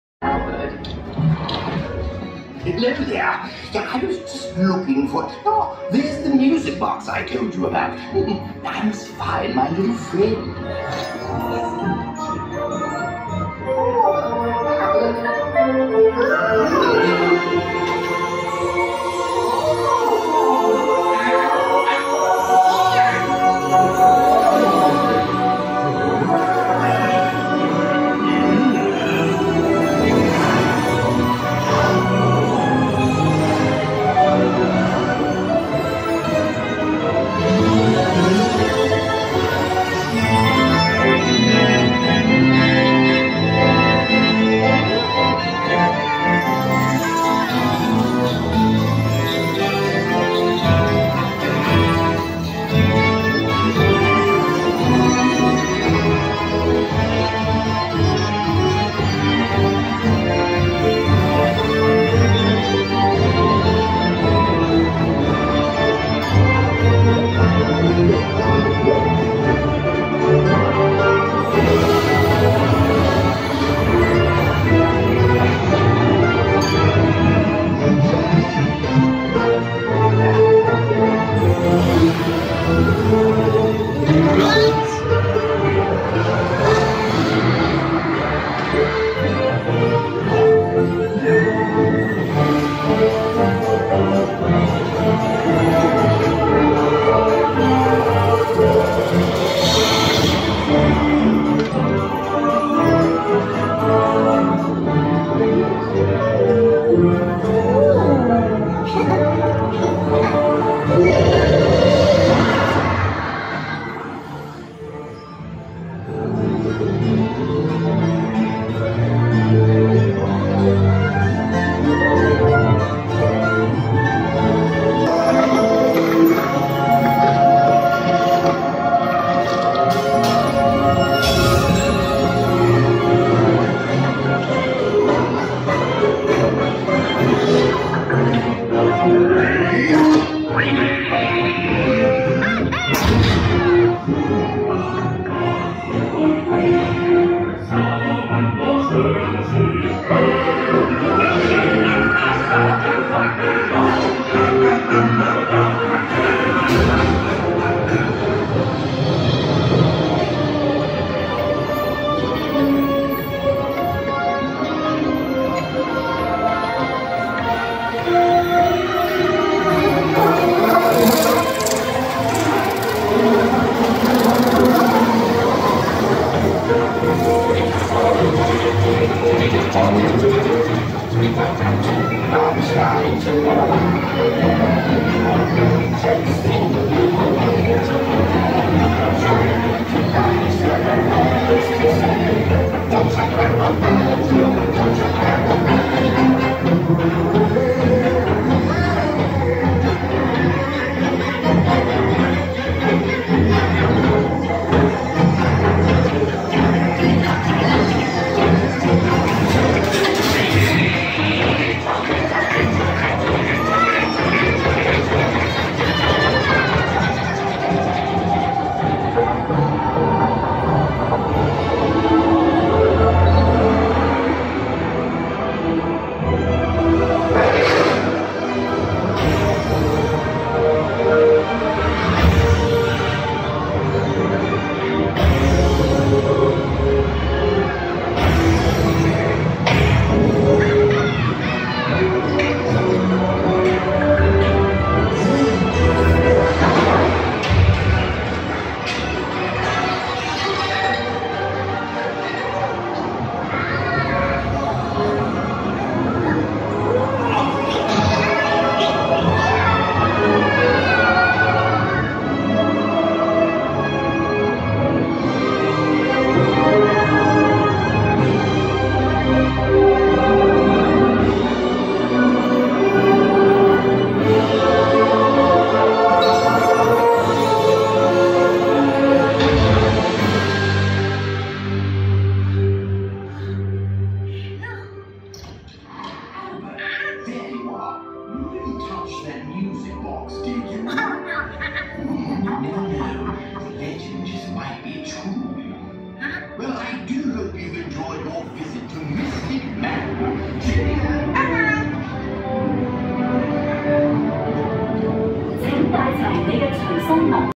Disneyland Most Underrated Ride - Mystic Point!
I've been to HK Disneyland more than 15 times. All my friends said this attraction is so so, hence I skipped it in my last 15 visits. Finally, I made it here because of Halloween and I found it somewhat delightful. It's definitely worth visiting!